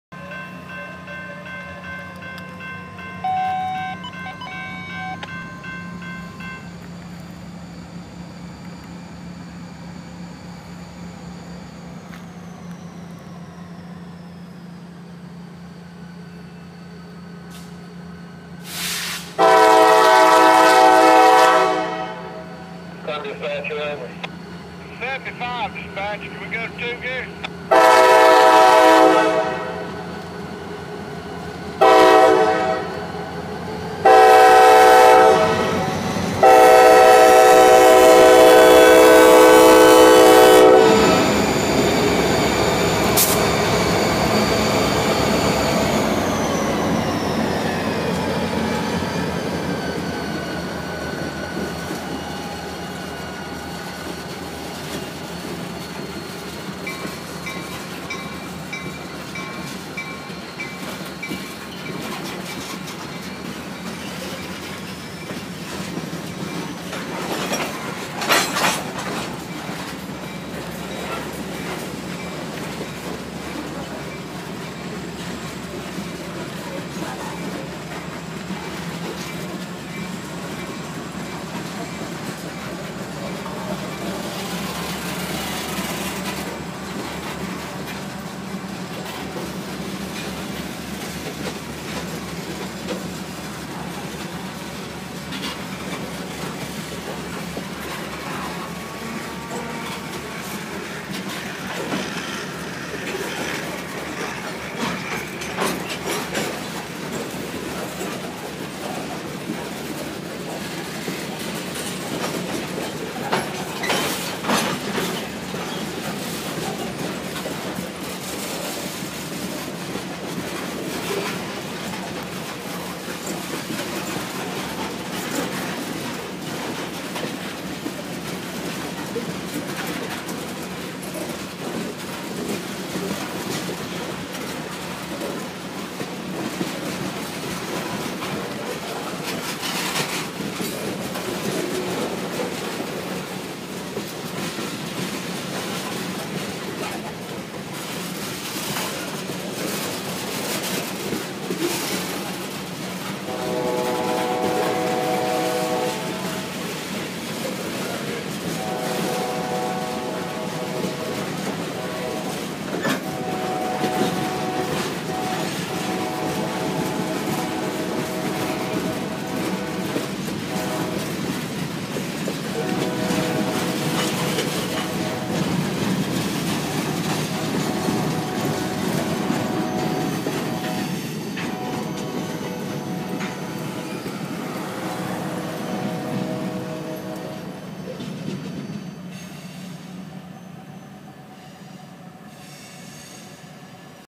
NS P75 crosses Devine Junction in Columbia SC
As the heat wave in Columbia increases, Columbia dispatch gives the all clear for NS P75 to cross Devine Junction and head out onto the line to switch out some cars. While I was on my way to work, I heard that switch 2402 got put into the normal position. Then in no time at all, I saw P75 coming around the corner w/ his SD40 lushup. Enjoy!
Lead unit: NS 3461 ex-CEFX SD40-2
Recorded 7/24/17
2:07 pm